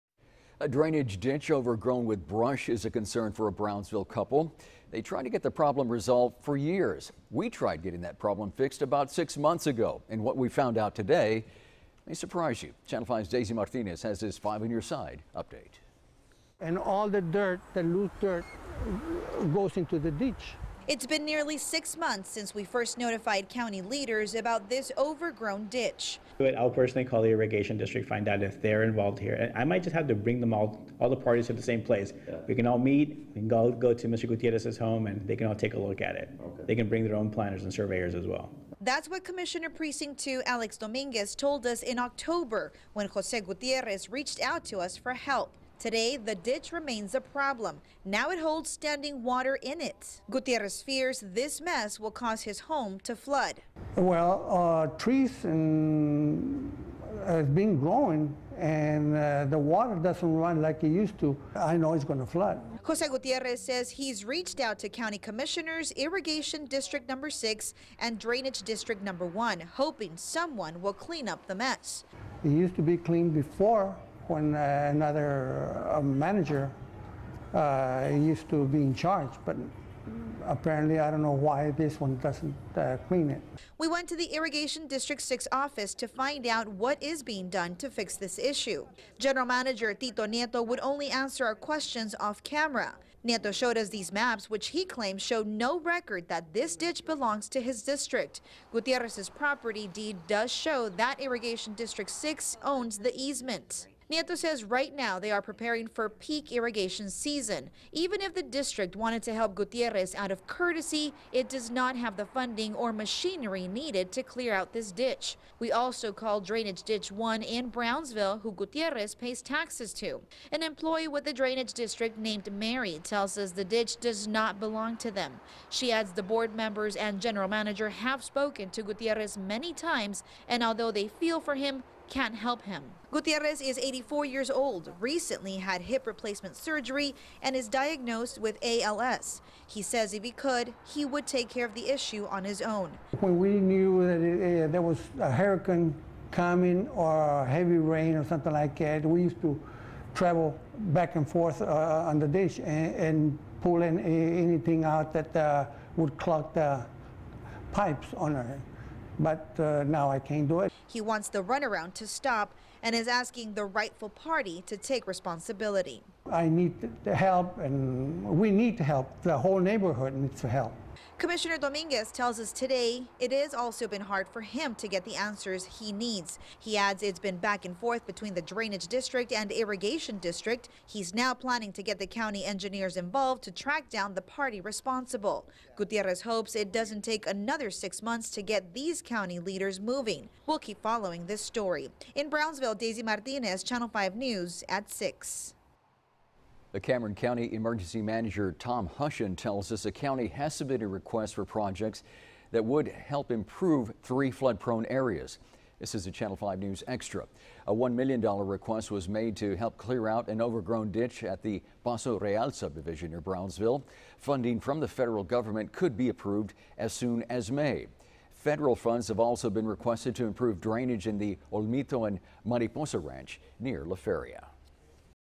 Brownsville Man Still Waiting for Drainage Ditch to be Clear
The problem with the ditch has persisted for years.